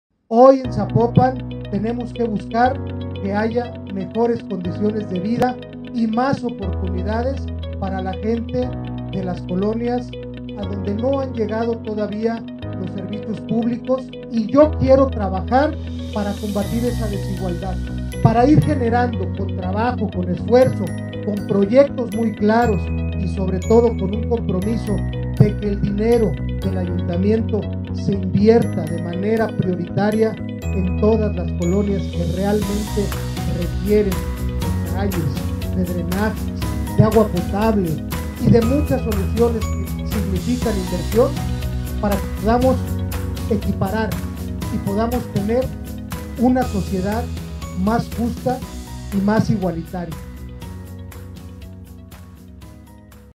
Sembrar la semilla del progreso - Abel Salgado
Es tiempo de sembrar la semilla del progreso y el avance social, hay que generar oportunidades para los zapopanos para tener un municipio más equitativo. EstoyContigo - Abel Salgado